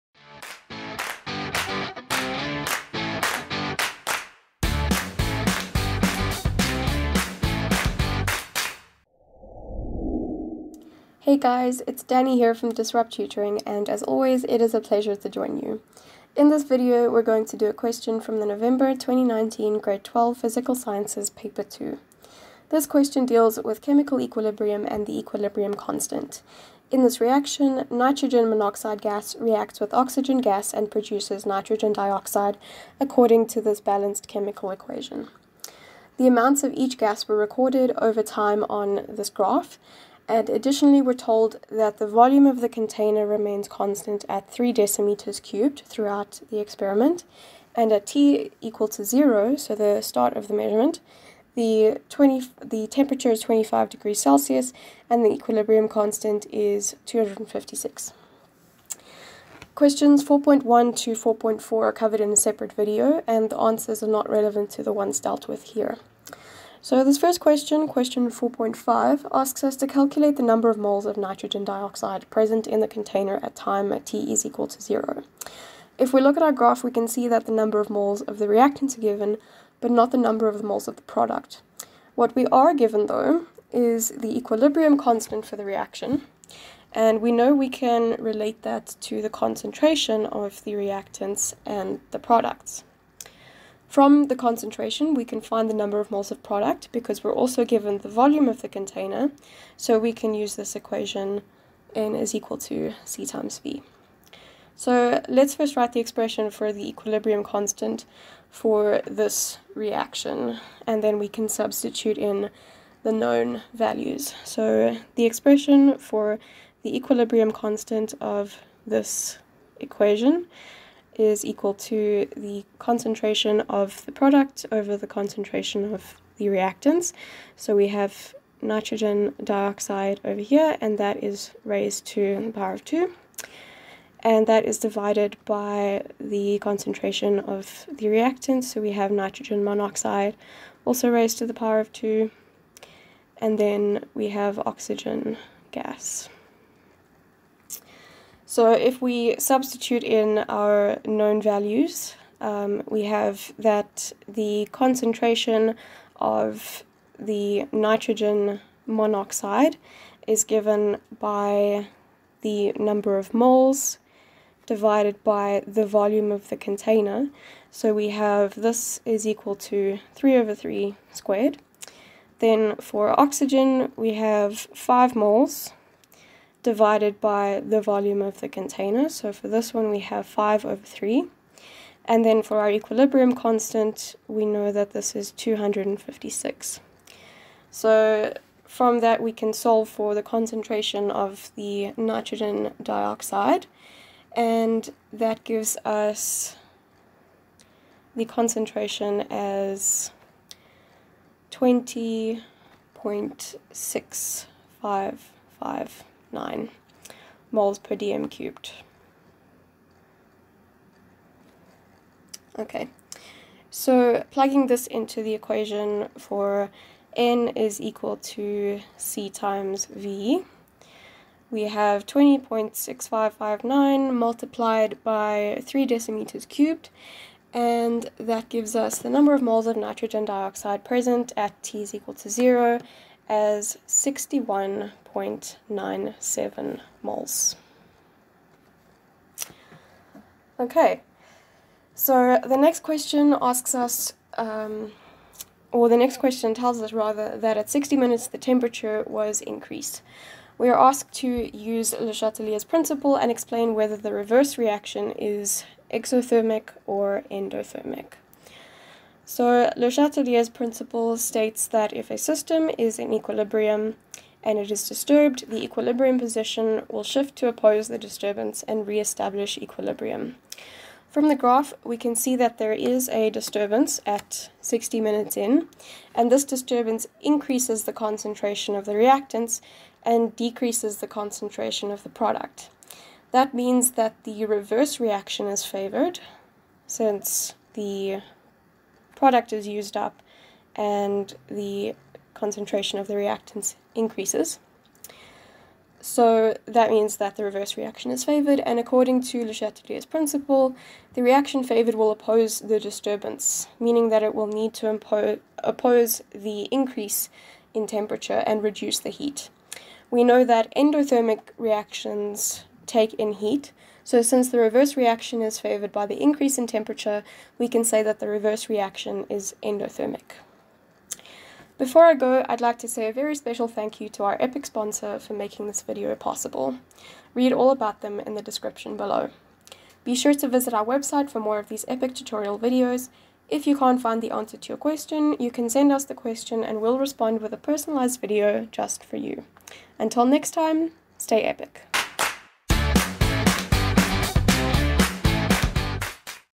#732 The equilibrium constant and Le Chatelier's principle
This question was expertly tutored by Danielle Sabatta.

The question covered in this tutorial video was sourced from a past paper (IEB 2019 Grade 12).